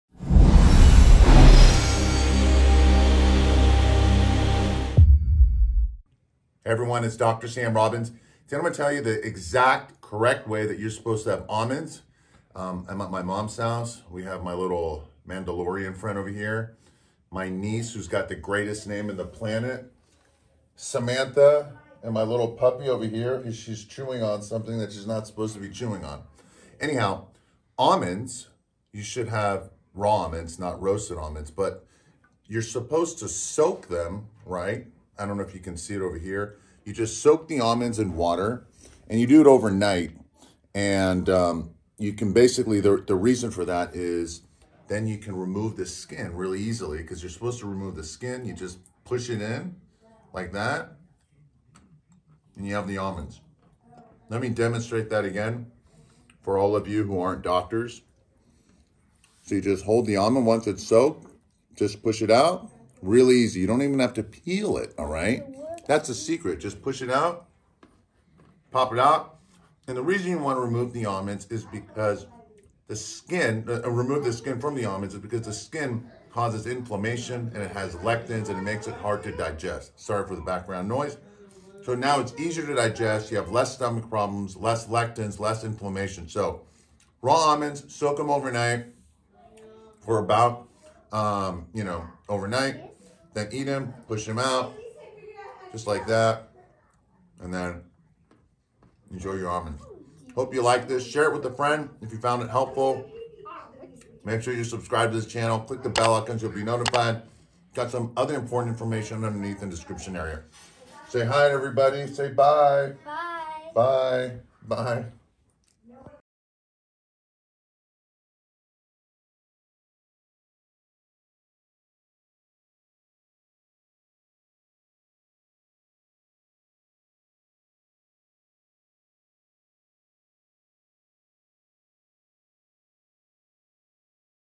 Never Eat Almonds Unless You Do THIS!
In this video you'll see what is the healthiest way of eating almonds!

Thanks
DrSamRobbins